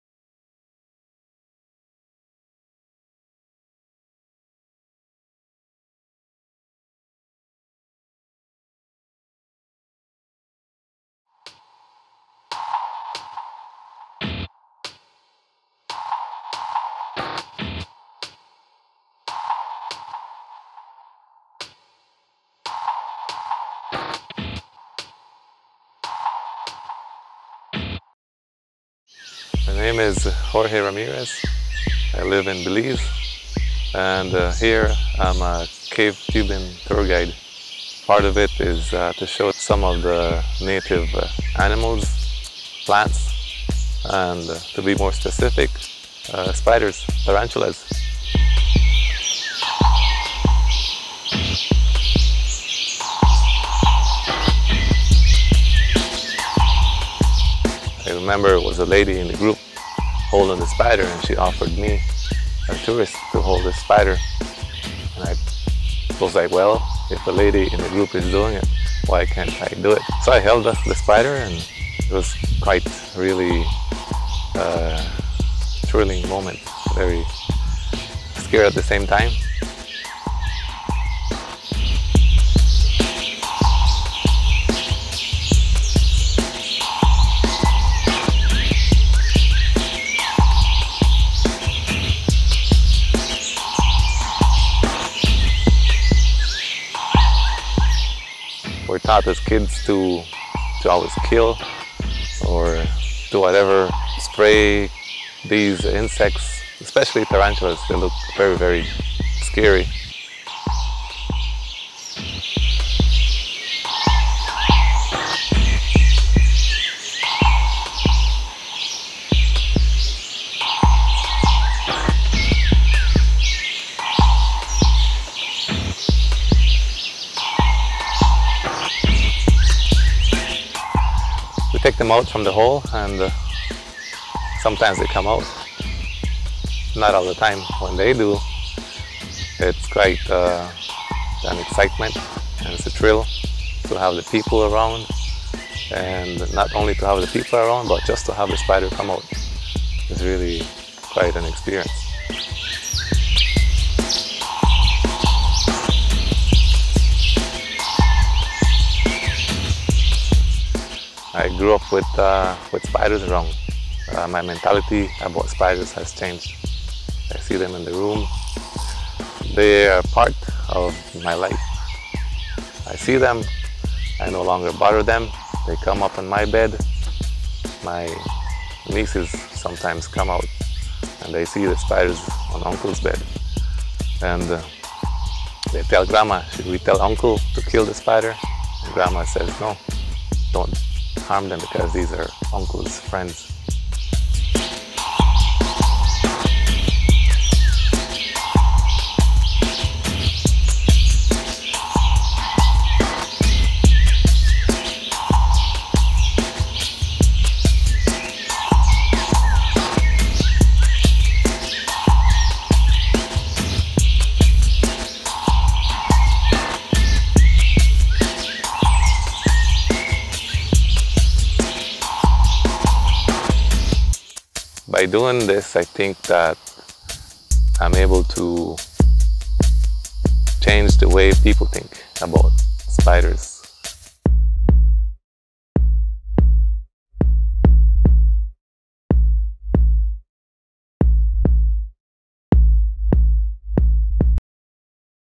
Spiderman and his Tarantulas in the Jungle of Belize
Jorge Ramirez alias Spiderman is showing his passion. He knows the hidden secrets of the jungle and loves the challenge of every adventure with these creatures. But it stays also for him always a great experience and the heart is still pumping, everytime when he is playing with his best friends, the big spiders of Belize: the Tarantulas